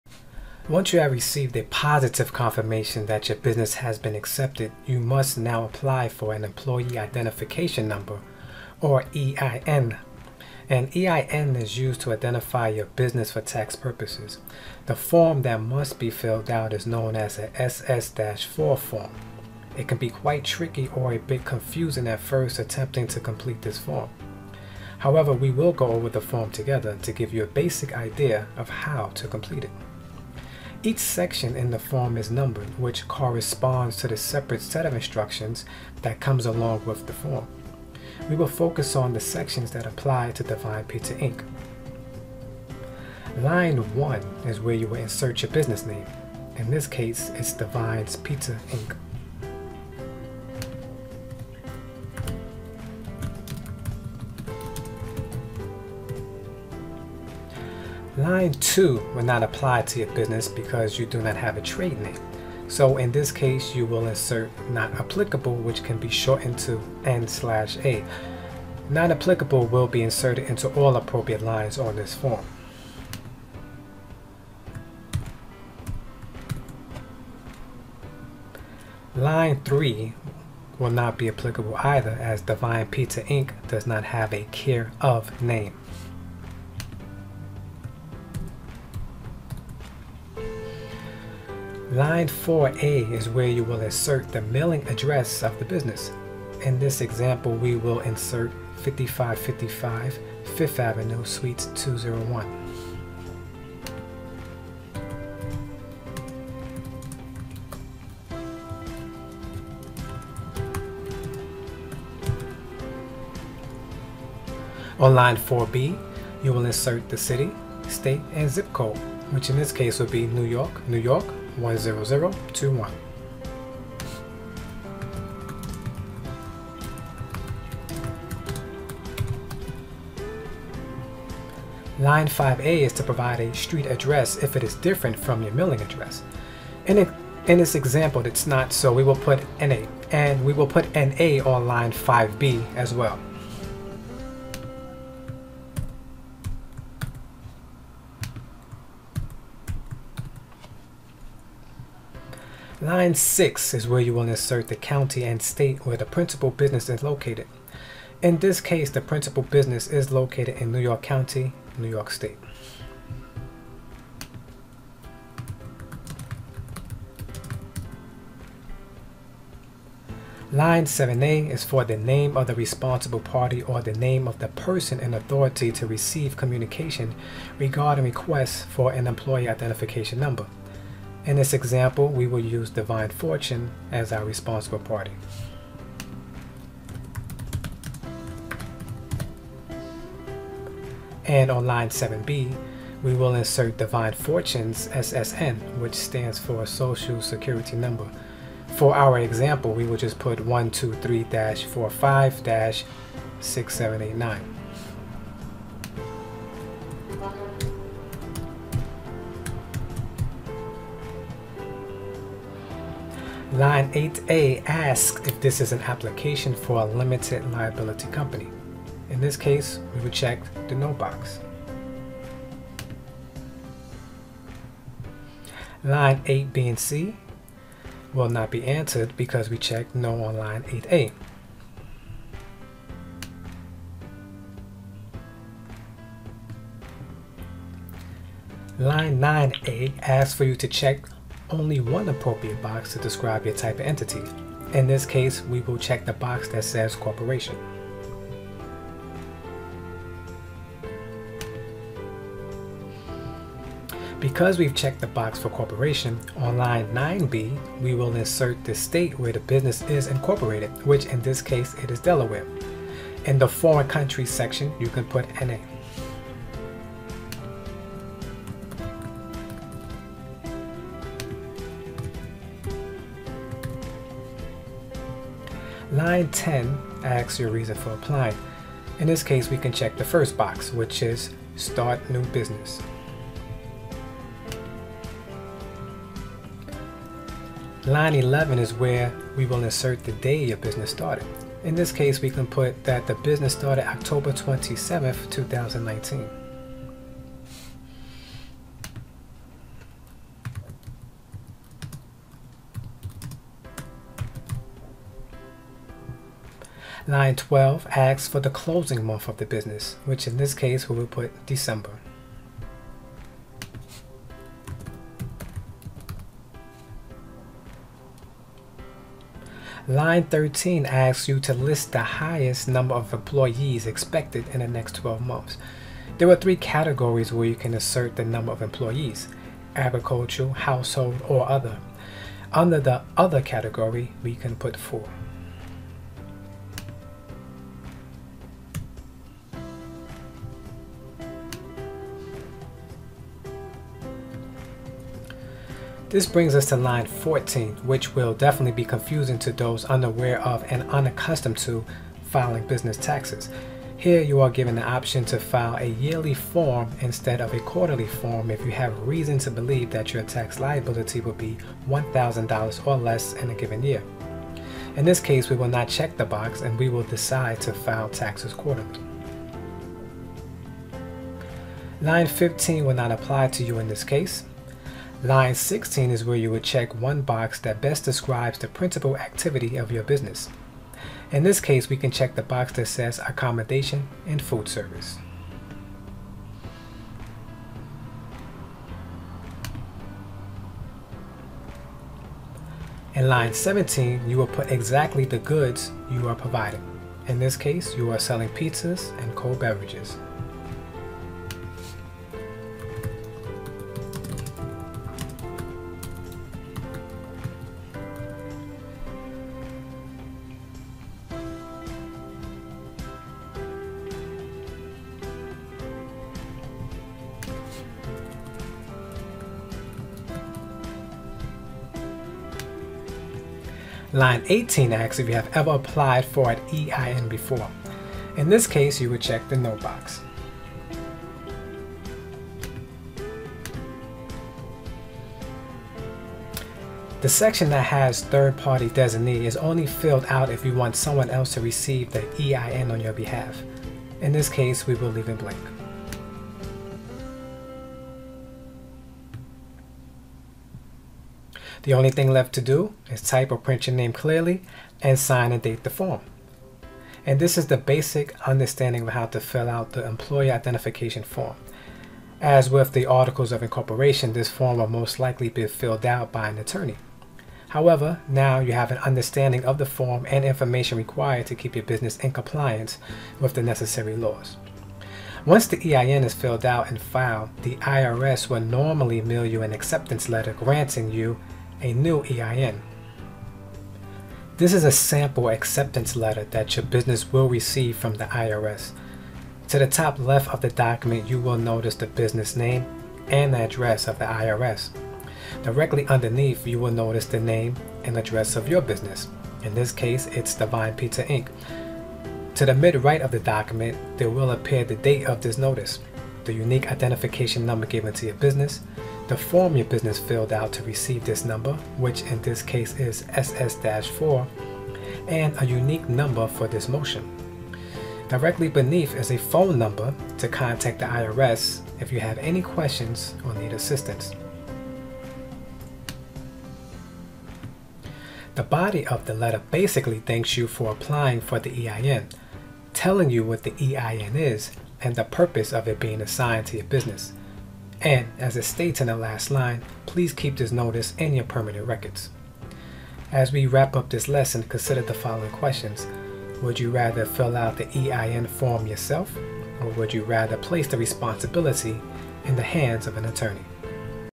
YEP - Employer Identification Number (EIN)
Learn what is an EIN and how to fill out the proper form.